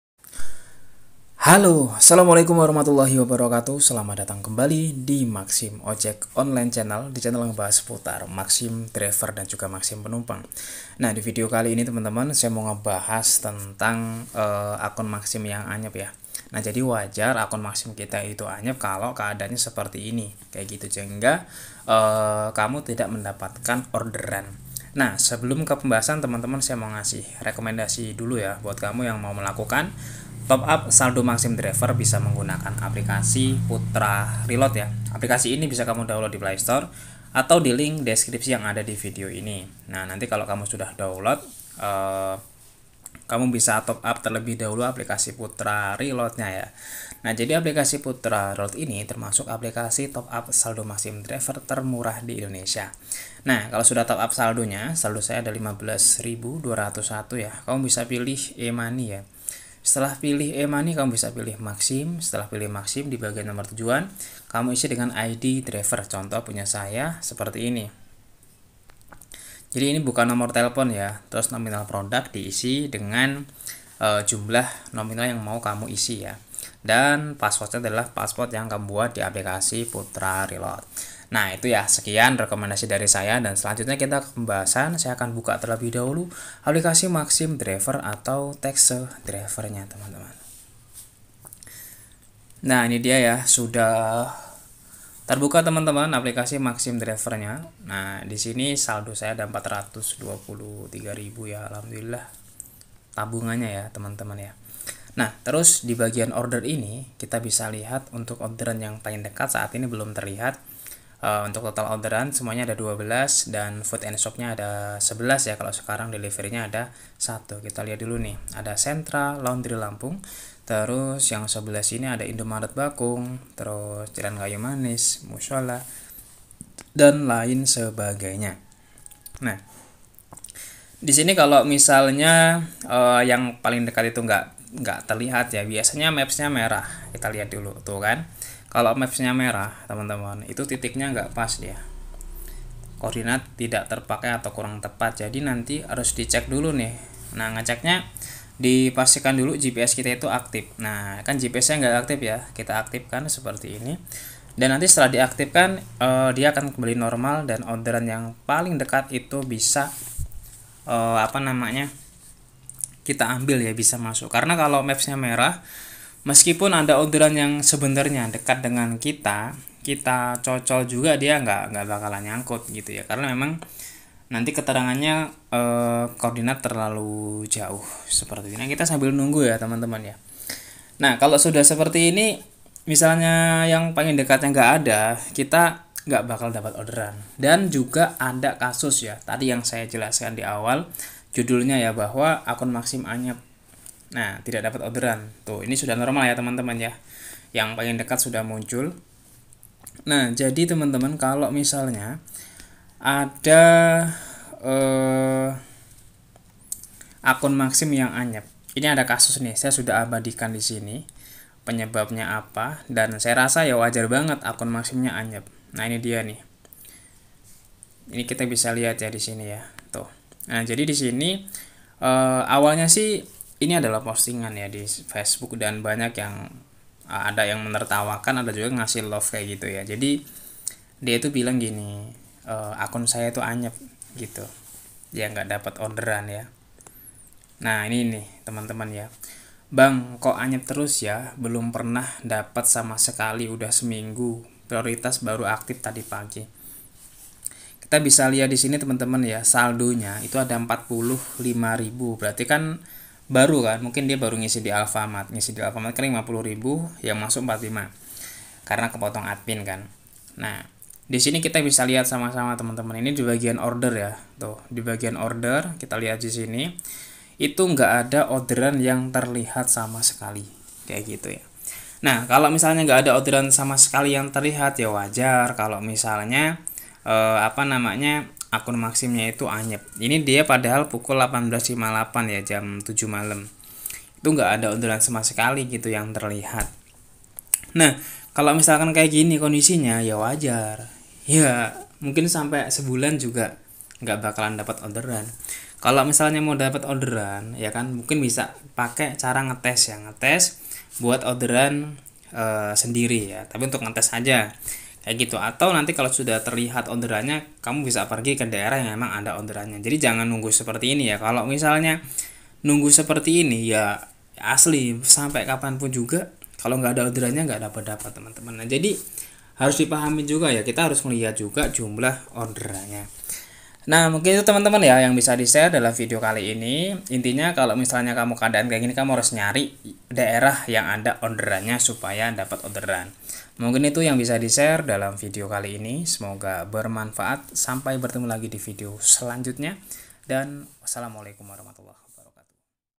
Akun Maxim Anyep..!! Akun Maxim Tidak Dapat Orderan..!! Ternyata Ini Masalahnya~Maxim Ojek Online
Halo semua, dalam video kali ini saya akan berbagi tentang akun maxim yang anyep. akun maxim anyep tidak dapat orderan bisa saja pernah kamu alami. saat akun maxim kamu anyep dan tidak ada orderan ada beberapa faktor yang menjadi penyebabnya. Nah berikut ini adalah salah satu penyebab akun maxim anyep yang di alami oleh seorang driver. kita simak video nya ya...

O iya bagi kamu yang mau download aplikasi putra reload bisa download aplikasi di sini ya..

salam..